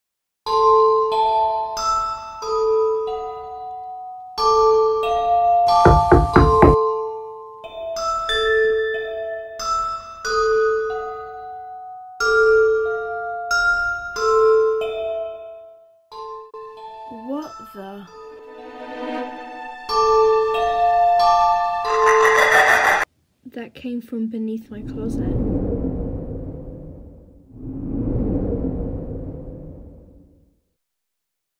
The Note | Beanie Boo Mini Horror Film | ((⚠️TW: 13+ /Flashing Lights))
We hope you enjoyed this short movie! Next week will bring a longer formatted video for y’all! 💫

This video Includes flashing lights and scary scenes which could be distressing for younger viewers. 13+ Audience recommended!